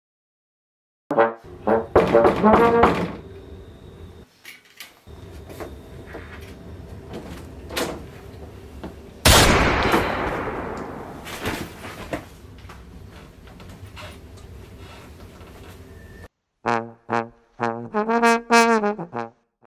My Worst Critic...
Very short film made as part of my editing for digital media class.